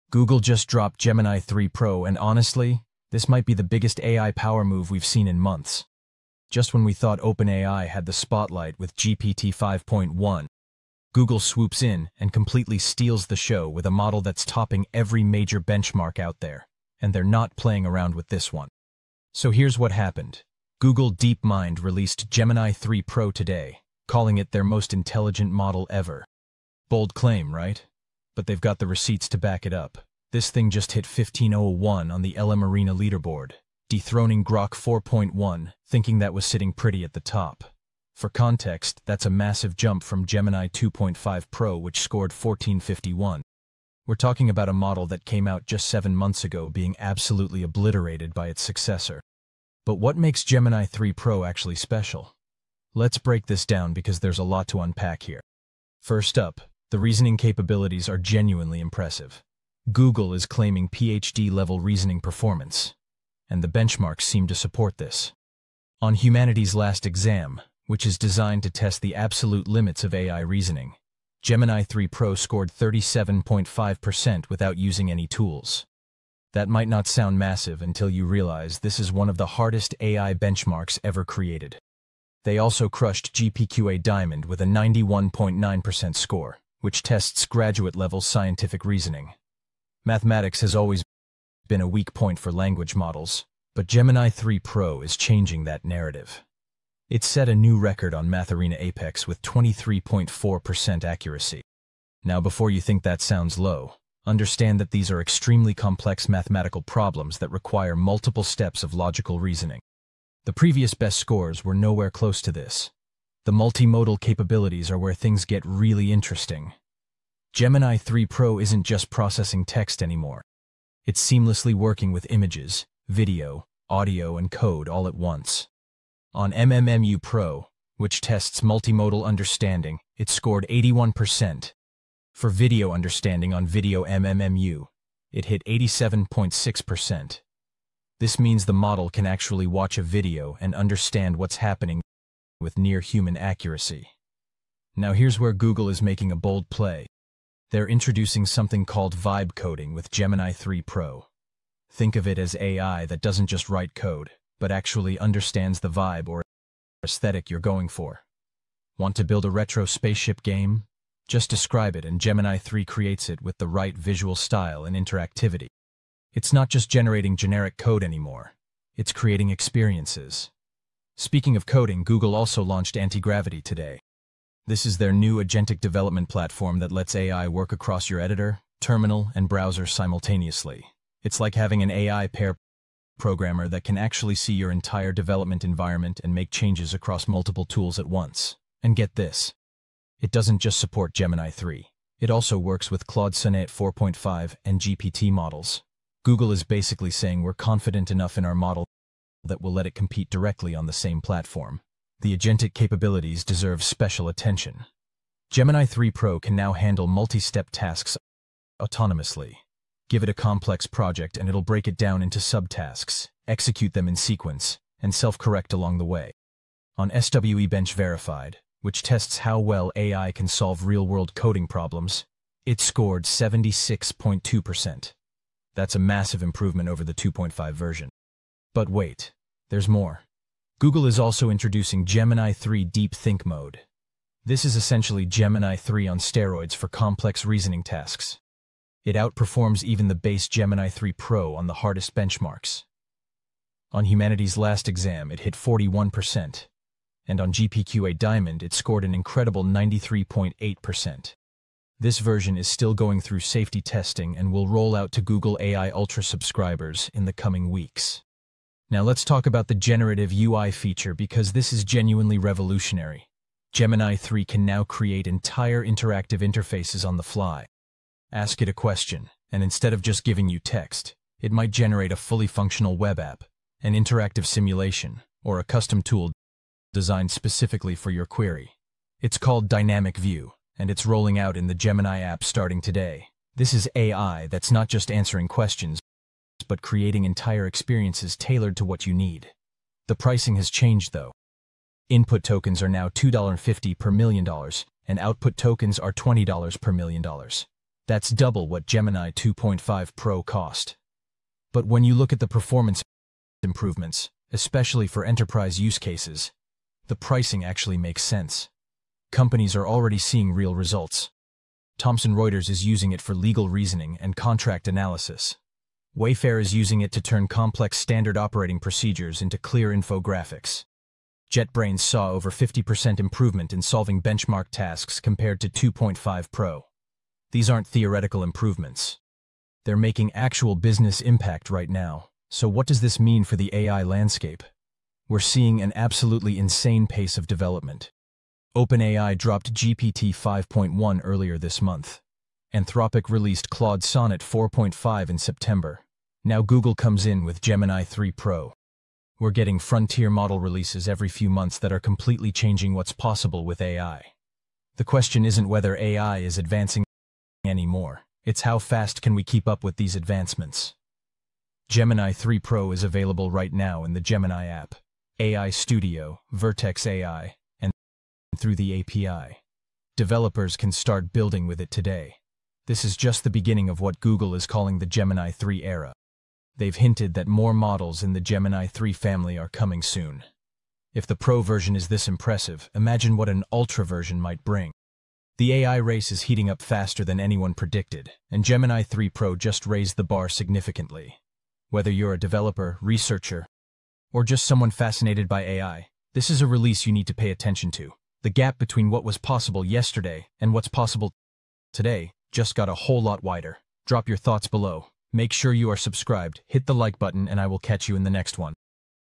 Google Gemini 3 Pro Released - The #1 Thing You Need to Know
Google DeepMind just released Gemini 3 Pro - their most intelligent AI model yet, and it's absolutely dominating the AI landscape! In this video, we break down everything you need to know about Google's latest breakthrough that's topping LMArena leaderboards and crushing GPT-5.1 and Grok 4.1.

What's Covered:
Gemini 3 Pro release details and availability
Record-breaking benchmark scores (1501 LMArena, 91.9% GPQA Diamond)
PhD-level reasoning capabilities explained
Revolutionary multimodal understanding (text, image, video, audio, code)
Vibe coding and generative UI features
Google Antigravity platform for agentic development
Gemini 3 Deep Think mode preview
Real-world enterprise use cases
Pricing and how to access it today

Key Benchmarks: ✅ LMArena: 1501 (dethroned Grok 4.1) ✅ Humanity's Last Exam: 37.5% (PhD-level reasoning) ✅ GPQA Diamond: 91.9% ✅ MathArena Apex: 23.4% (new record) ✅ SWE-bench Verified: 76.2% ✅ MMMU-Pro: 81% ✅ Video-MMMU: 87.6%

Gemini 3 Pro is available NOW in the Gemini app, AI Studio, Vertex AI, and through the Gemini API. This release comes just days after OpenAI's GPT-5.1 launch and shows Google is serious about dominating the AI space.

Timestamps:
0:00 - Intro: Gemini 3 Pro Announcement
0:45 - Breaking Benchmark Records
1:30 - PhD-Level Reasoning Explained
2:15 - Multimodal Capabilities
3:00 - Vibe Coding & Generative UI
3:45 - Google Antigravity Platform
4:30 - Gemini 3 Deep Think Mode
5:15 - Enterprise Use Cases & Pricing
6:00 - What This Means for AI Future

About AI Perspectives: We cover the latest breakthroughs in artificial intelligence, machine learning, and emerging tech.